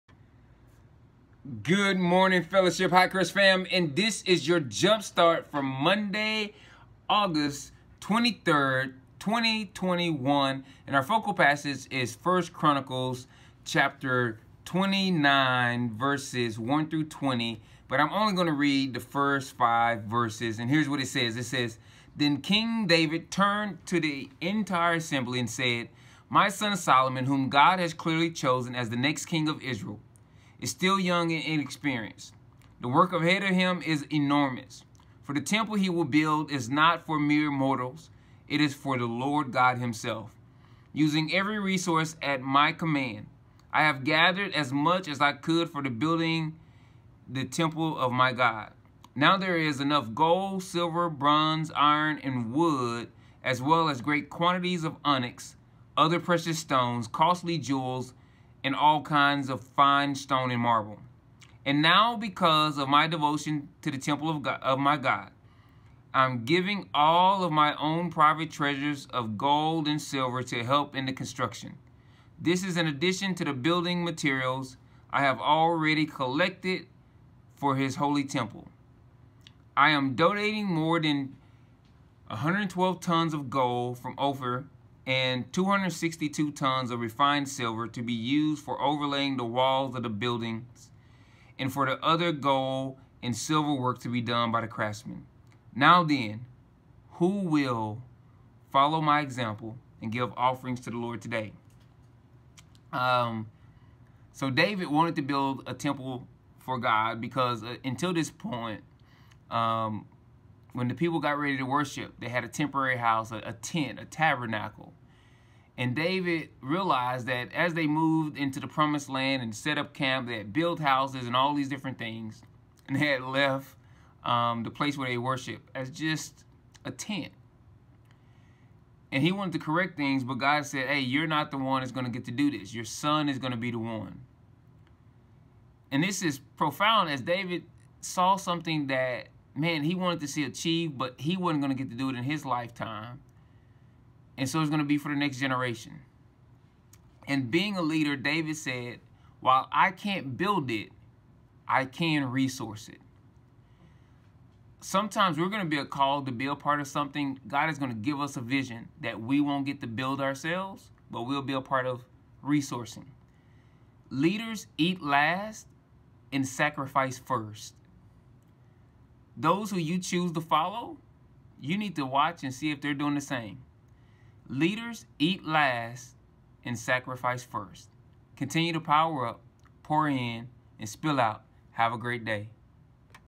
Jumpstart your day! 1 Chronicles 29:1-20
Check out today's Jump Start by Johnathan Sublet! Our focal passage today is 1 Chronicles 29:1-20.

Fellowship Hi-Crest | Walking in community with others through the power of Jesus Christ